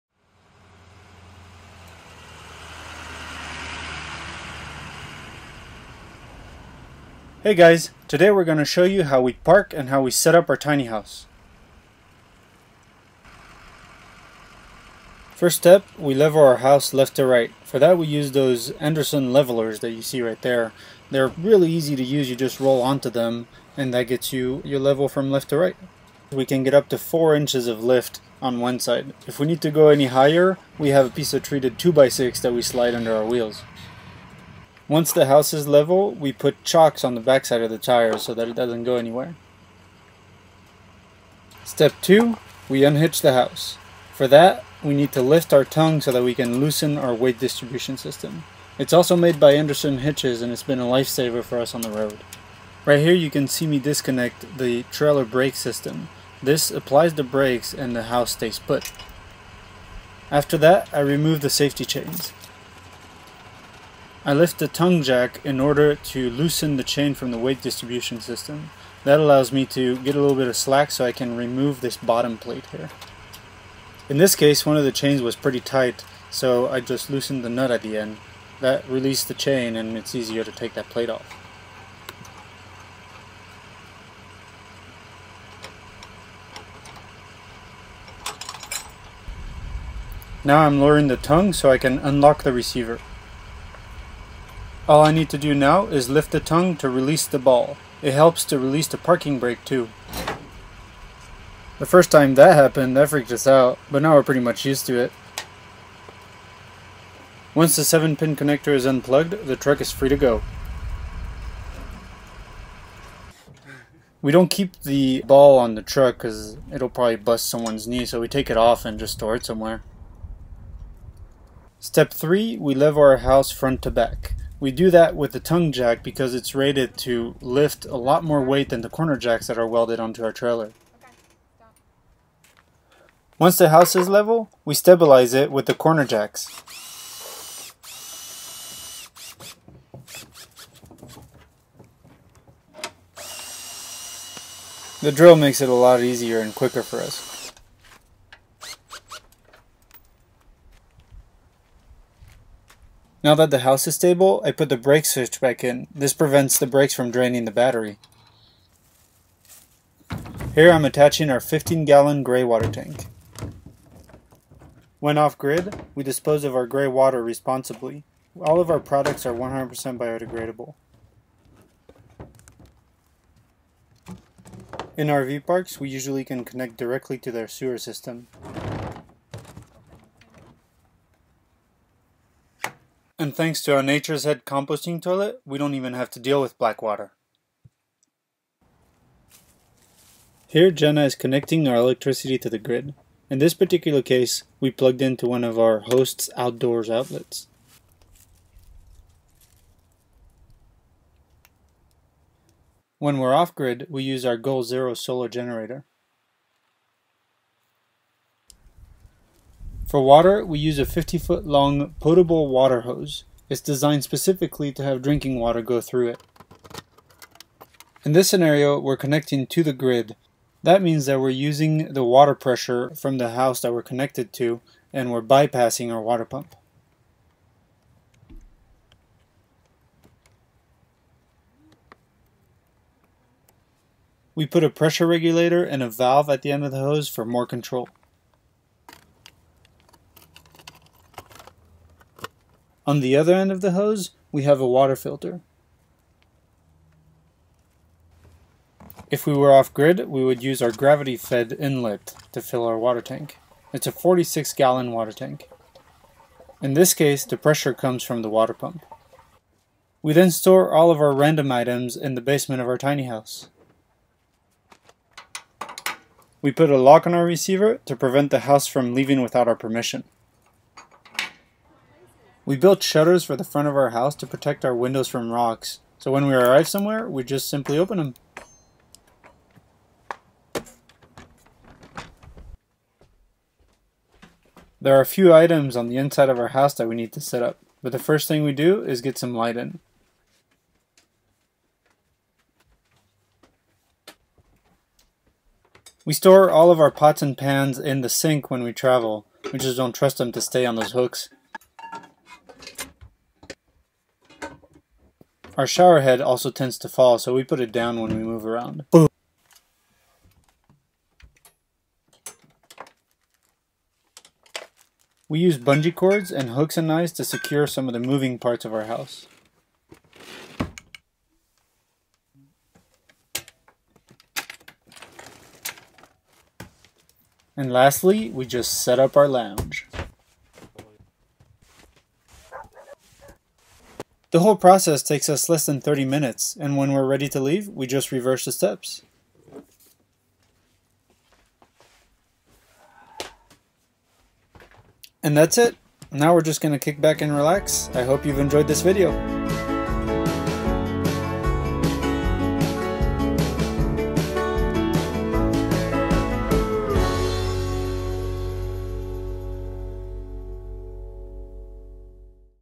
How to Park a Tiny House and Hook Up to Electricity and Water
Ever wondered what it takes to park and set up a tiny house? In this 6 minute video we show you how we do it in 10 easy steps.

Interested in the products used in this video, here are details (in order of appearance):

Step 1: Level Left & Right
- Andersen Levelers and Chocks
- Graduated Bubble Levels

Step 2: Detach The House
- Andersen No-Sway No-Bounce Weight Distributions System

Step 3: Level Front & Back

Step 4: Stabilize
- Andersen Tough Pads
- Milwaukee Drill

Step 5: Connect Grey Water
- 15 Gal Thin Grey Water Tank
- 3' Sewer Hose
- 15' Sewer Hose
- Sewer Blade Valve
- Nature's Head Toilet

Step 6: Connect Electricity
- Marinco 30 Amp Inlet
- Marinco 30 Amp / 15 Amp Pigtail Adapter
- Heavy Duty 15 Amp Extension Cord
- Goal Zero Yeti 1250 Solar Generator
- Two 100W Renogy Solar Panels

Step 7: Connect Water
- 50' Drinking Water Hose
- Hose Caps
- Water Inlet
- Stainless Steel Regulator
- Water Hose Valve: Home Depot
- Water Filter
- 46 Gal Water Tank
- Water Pump

Step 8: Exterior Set Up
- Receiver Lock

Step 9: Interior Set Up
- Curtain Tension Rods
- Propane Cooktop
- Low Flow Showerhead
- Shower Diverter
- Chrome Shower Hose
- Chrome Shower Bracket
- Dometic 3 Way Fridge
- Sliding Storage
- Ottomans (modified)

Step 10: Relax!

Here is a list of all other special materials we used